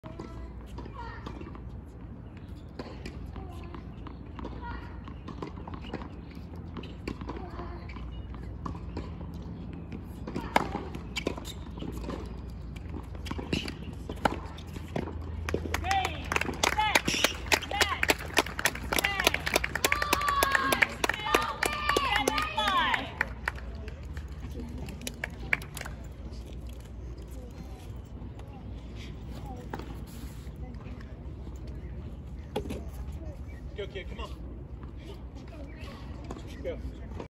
Easter Bowl B12s Final Match Point
Jiarui Zhang defeated Nathan Lee 6-2, 7-5, in B12s Easter Bowl final, March 28, 2024, Indian Wells Tennis Garden, Indian Wells California